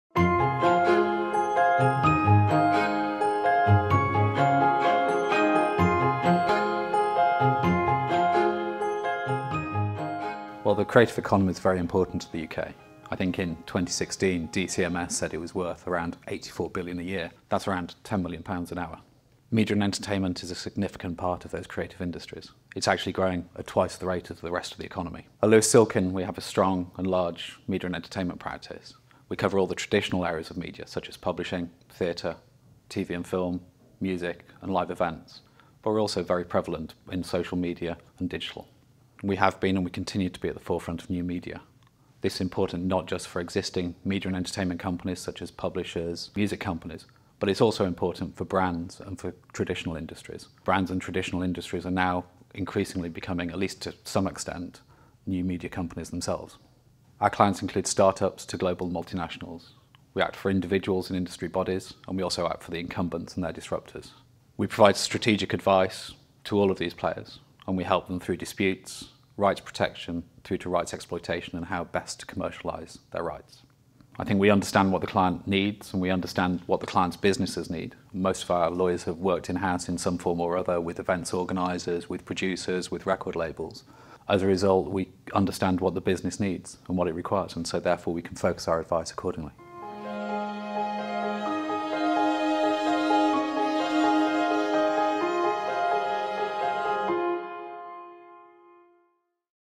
Lewis Silkin's Media & Entertainment Sector Introduction
Jeremy Summers, Head of Media & Entertainment at Lewis Silkin introduces our perspective and some thoughts on the Media & Entertainment sector.